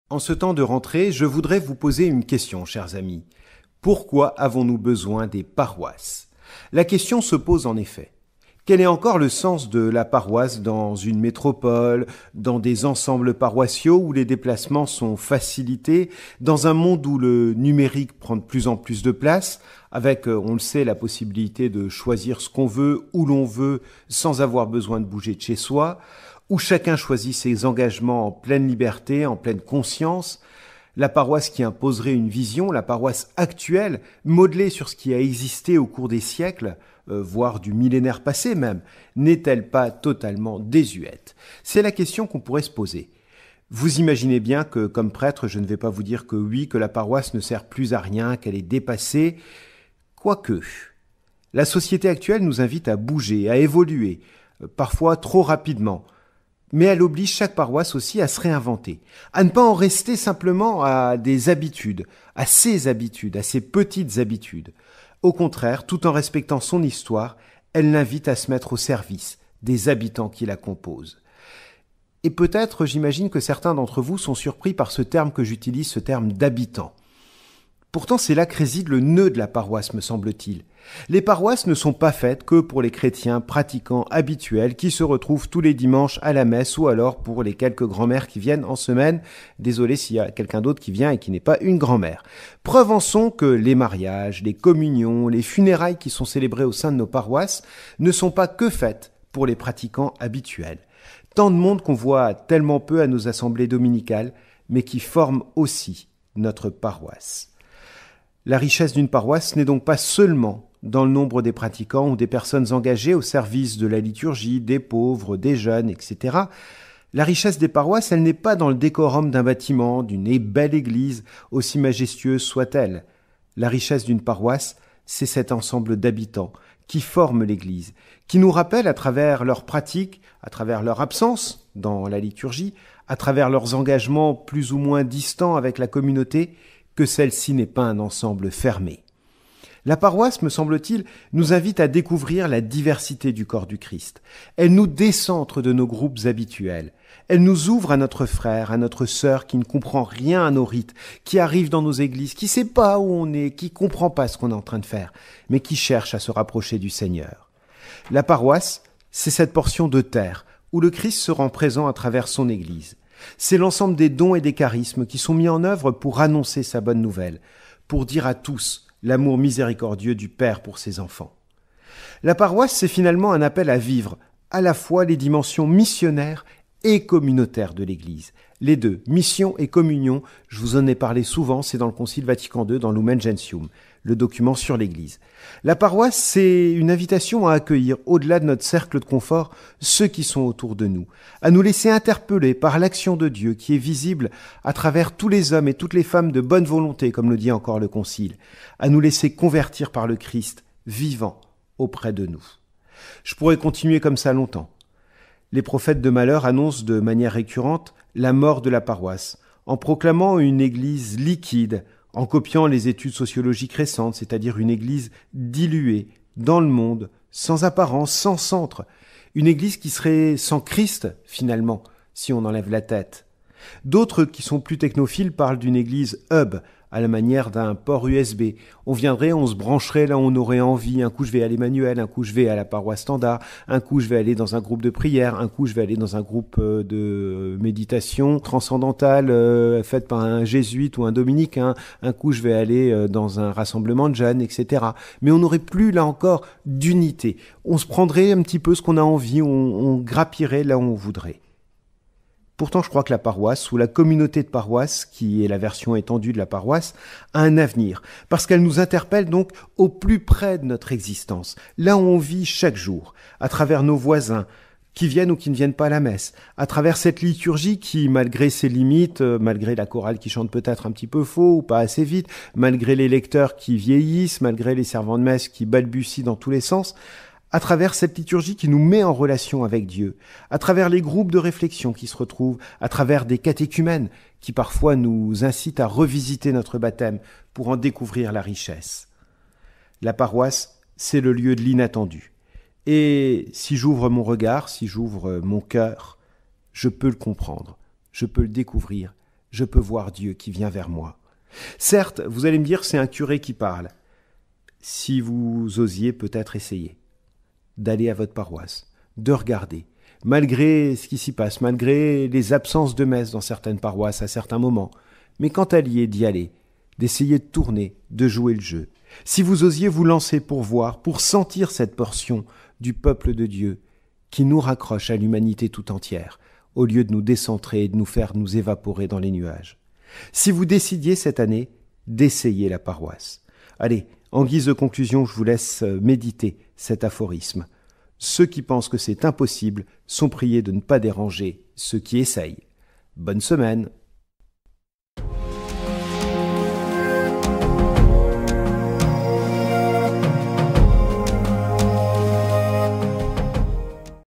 Plaidoyer pour la paroisse – Père Stéphane Lemessin
Cette semaine le Père Lemessin nous invite à réfléchir sur ce qu’est une paroisse, son intérêt, son sens, les alternatives à la paroisse. Les évolutions que pourraient connaître les paroisses. Retrouvez le père Lemessin chaque semaine sur Missionweb Radio